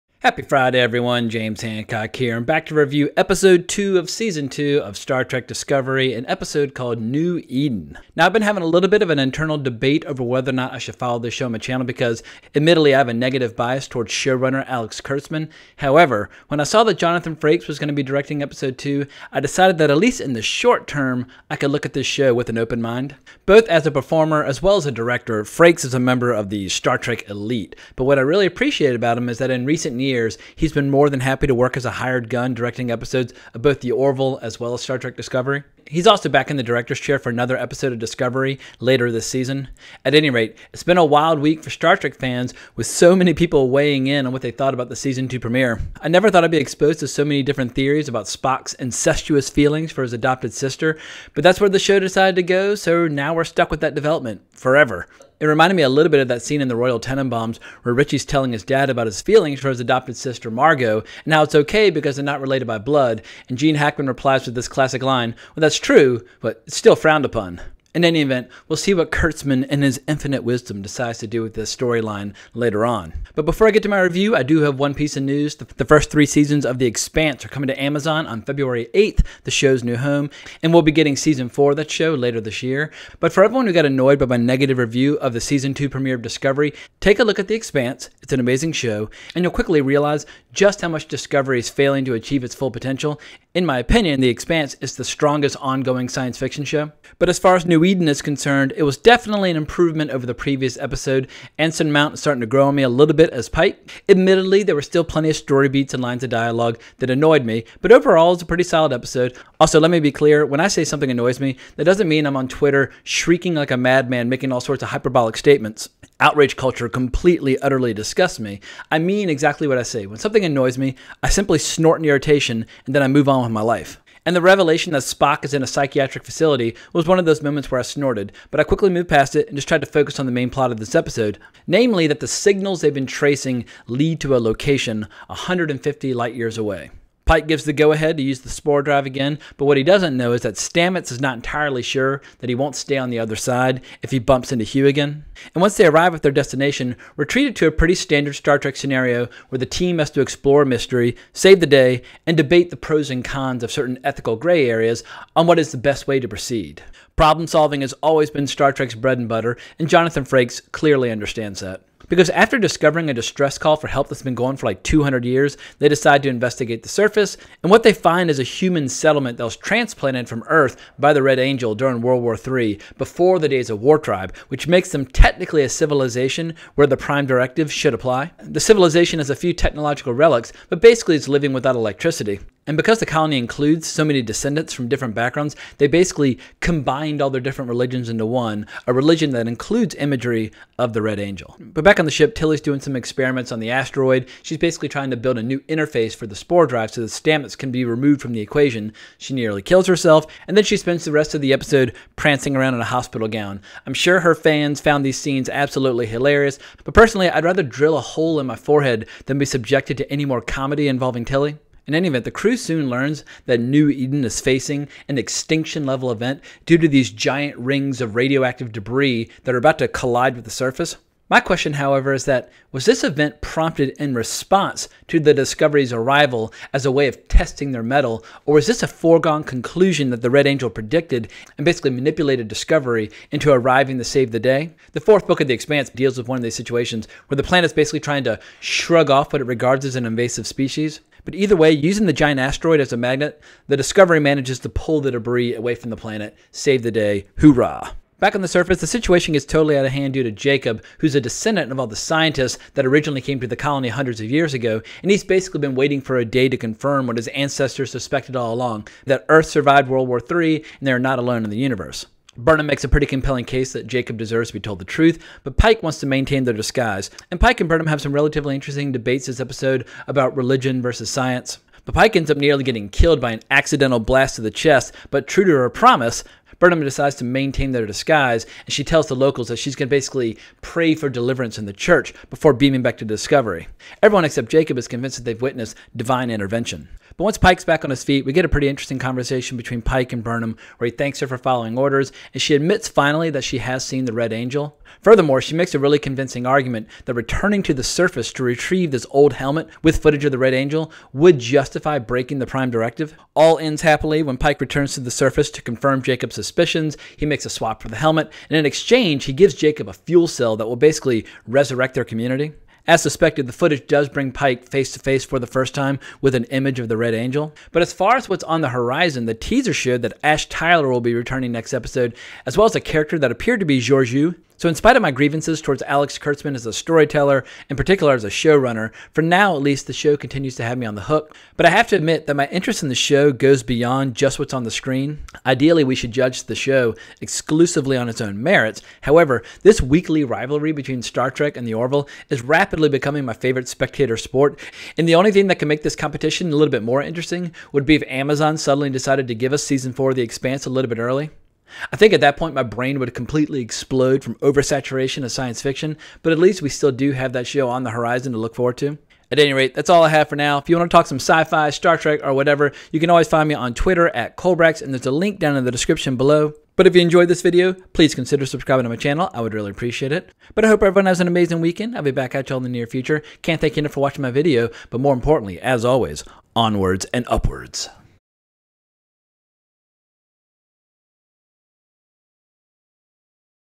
Star Trek: Discovery - Season 2 Ep. 2 "New Eden" Review
My review for episode 2 of season 2 of 'Star Trek: Discovery', directed by Jonathan Frakes.